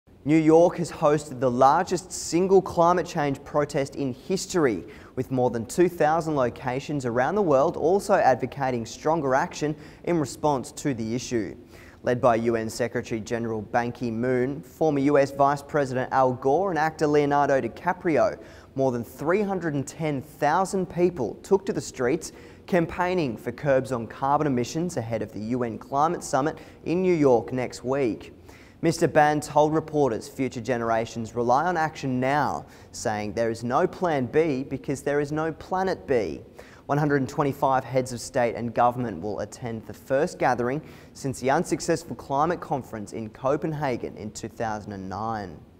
Thousands March For Climate Change - Sep 22, 2014
New York has hosted the largest single climate change protest, with more than 2000 locations around the world also advocating stronger action in response to the issue. Led by UN Secretary-General Ban Ki-moon, former US vice-president Al Gore and actor Leonardo DiCaprio, more than 310 000 people took to the streets campaigning for curbs on carbon emissions ahead of the UN climate summit in New York next week. Mr Ban told reporters future generations rely on action now, saying “there is no Plan B, because there is no Planet B”. 125 heads of state and government will attend the first gathering since the unsuccessful climate conference in Copenhagen in 2009.